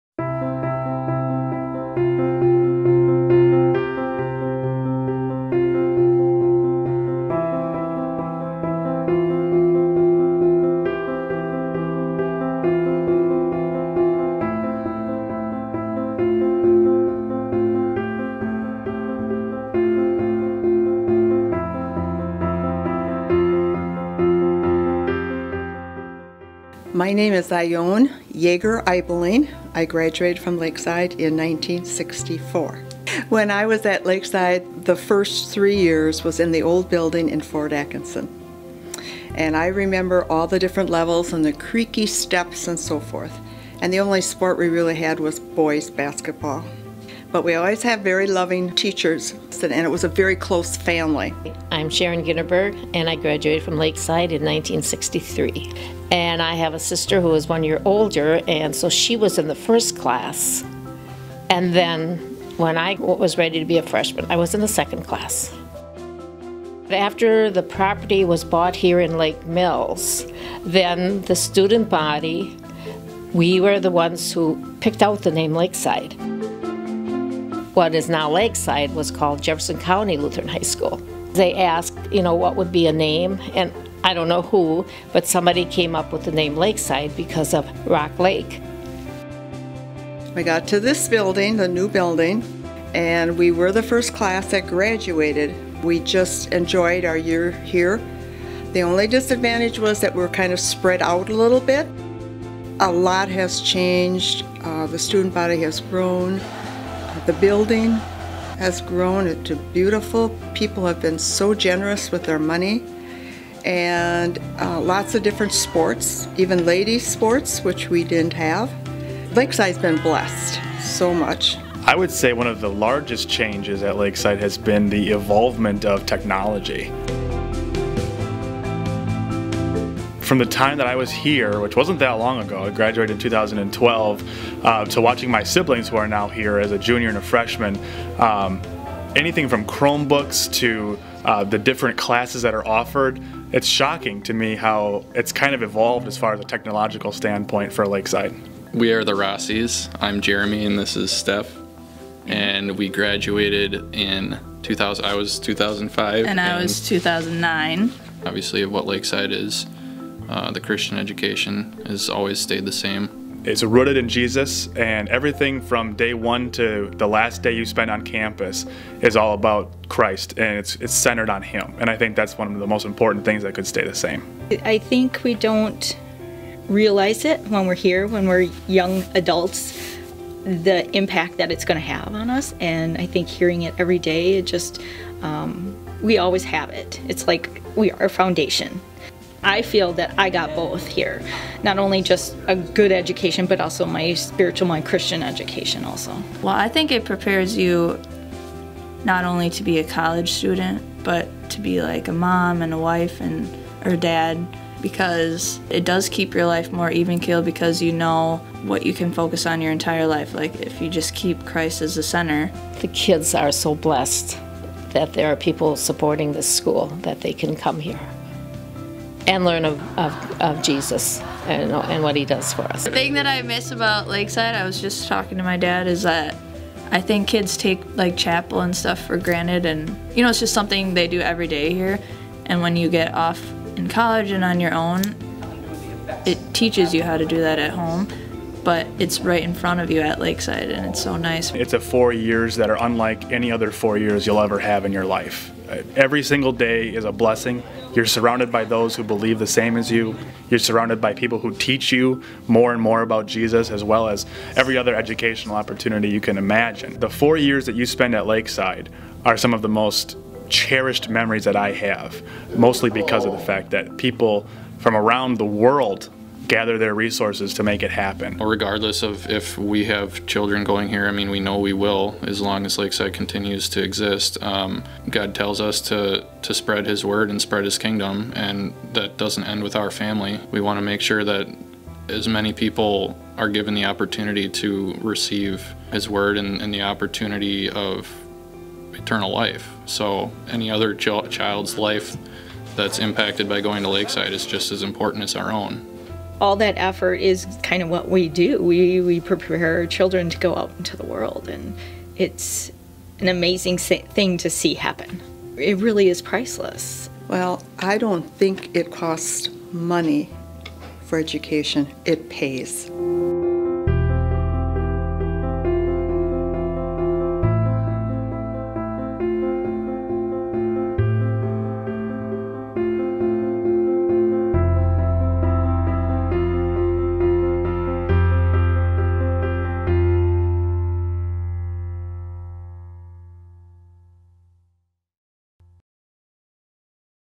Decades of Faithfulness
Lakeside celebrates 60 years of students growing in the knowledge of God's Word.